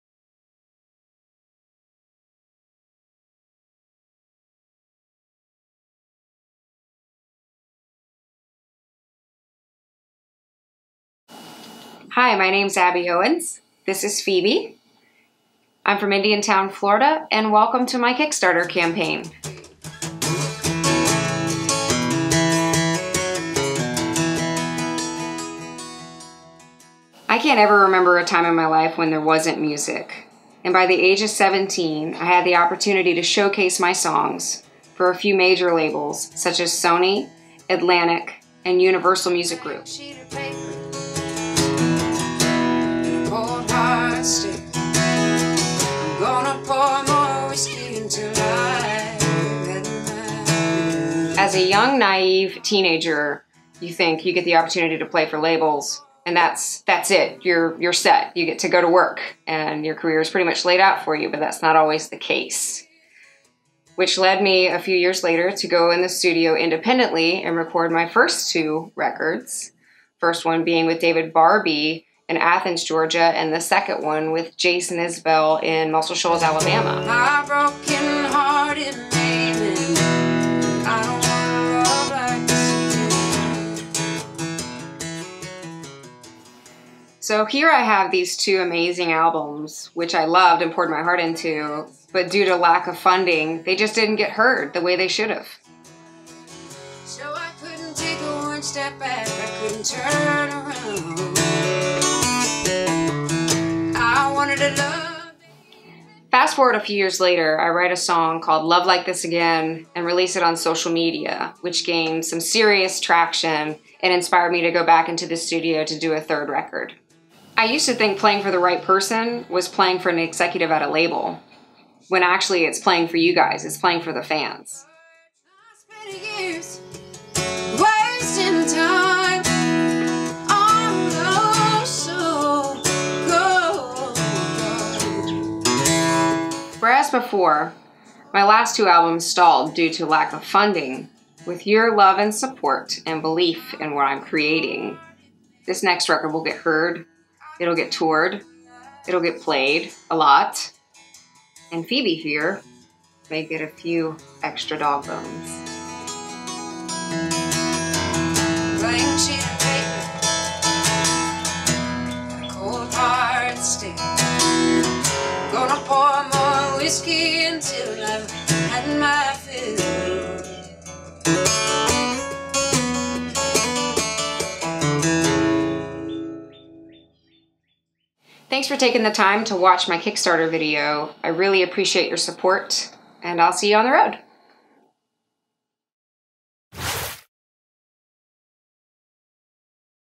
Abby Owens Kickstarter Campain Video - TightEdit
The following Kickstarter video was produced by Phil Galdys, owner of Sailfish Photography, LLC, for singer/songwriter Abby Owens.

The music segments of the video were filmed in Indiantown, FL, on the porch of an abandoned house of which was originally owned and occupied by Abby's grandparents. It was a place where she grew up and played as a child. The property is currently owned by King Ranch, who graciously granted us permission to shoot on location.

The interview segments of the video were filmed at Terra Fermata
 in Stuart, FL. The owners, who are always happy to support our local music talent, opened the building before regular business hours so that Abby and 'Phoebe the dog' could speak from the heart.